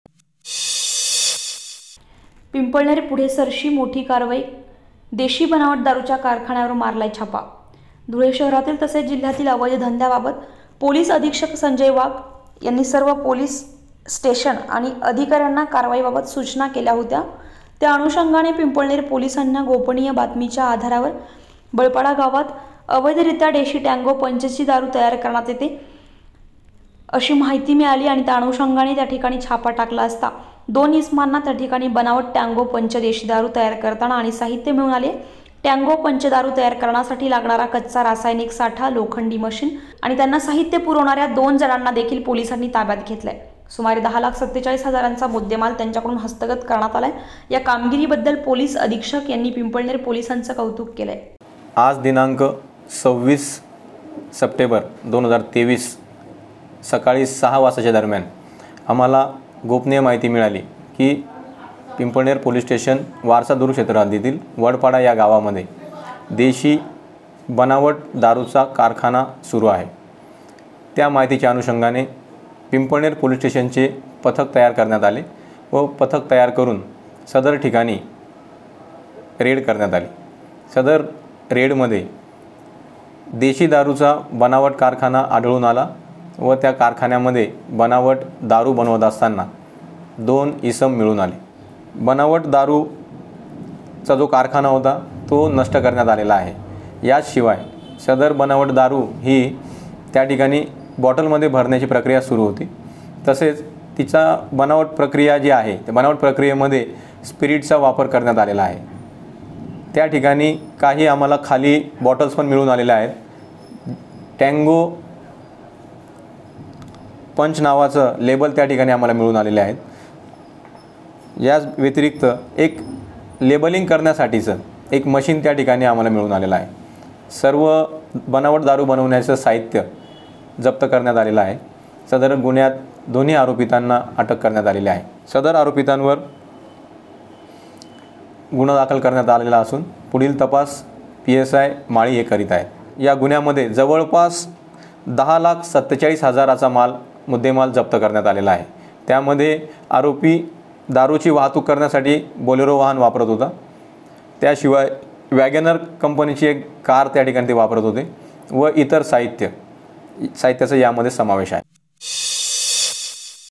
पिंपळनेर पुढे सरशी मोठी कारवाई देशी बनावट दारूच्या कारखान्यावर मारला छापा.
दहा लाख 47 हजारचा मुद्देमाल यांच्याकडून हस्तगत.